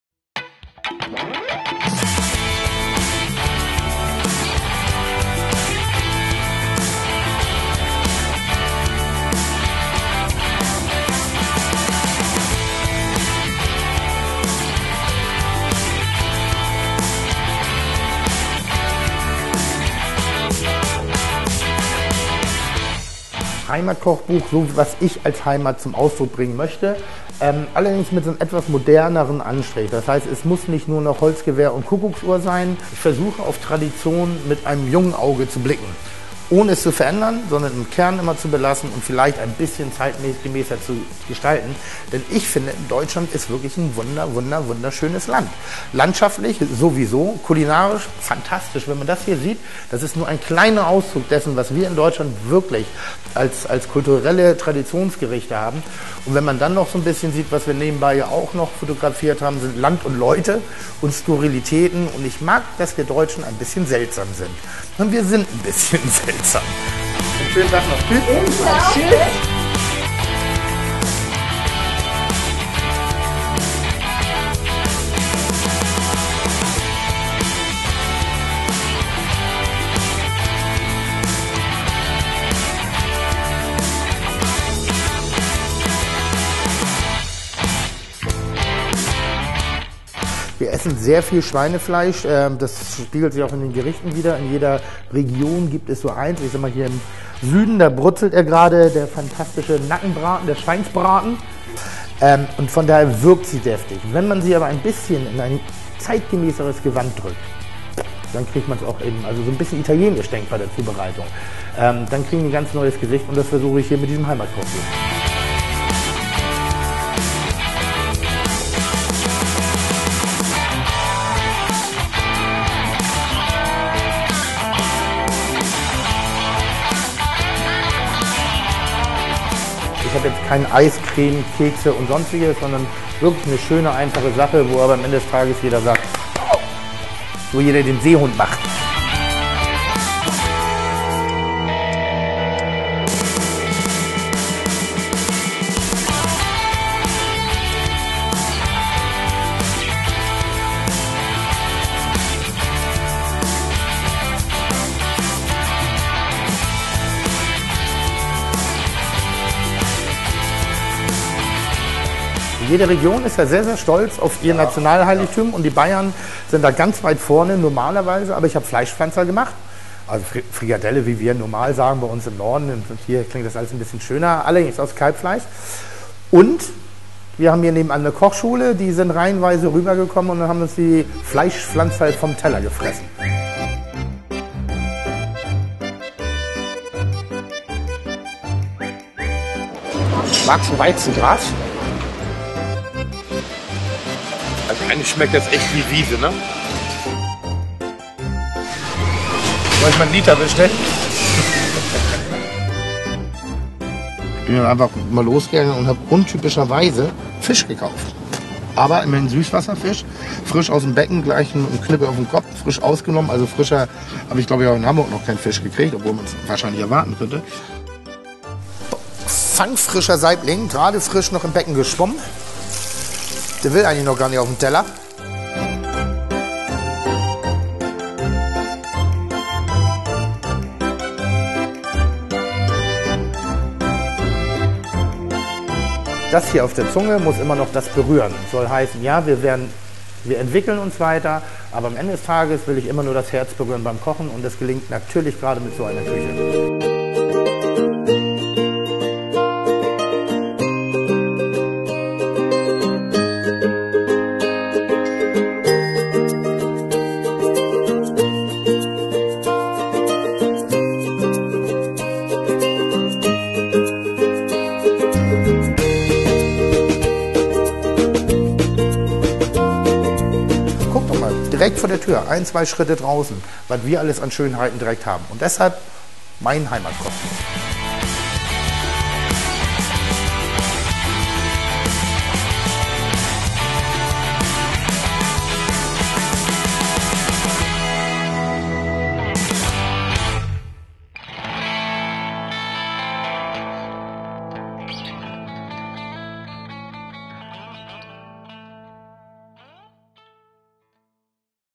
Making-Of (Langversion) zum Trailer von Tim Mälzers neuem Kochbuch HEIMAT
HEIMAT erscheint im Oktober 2014. Jetzt vorbestellen!

Heimat Deutschland - Bockwurst und Sauerkraut, Präzision und Lederhose? Tim Mälzers Blick auf seine Heimat ist anders und frei von Klischees! Für ihn ist Heimat überall, wo man sich zuhause fühlt: im Fußballstadion, bei der Familie, am Meer wie in den Bergen, beim Camping, zu Lande und zu Wasser … Wie frisch und kreativ deutsche Küche sein kann, zeigt der Autor mit umsichtig modernisierten klassischen Rezepten. Vertrauen auf beste Produkte: bunte Vielfalt an regionalen Gemüsen und Kräutern, Obst aus heimischem Anbau, bestes Fleisch, Fische aus Meer und Seen, Käse vom Hof. Dazu weltberühmte Weine und perlendes Bier – alles in Handarbeit. Ein Schlaraffenland. Frisch und unverbraucht, jung und modern: So schmeckt Heimat heute!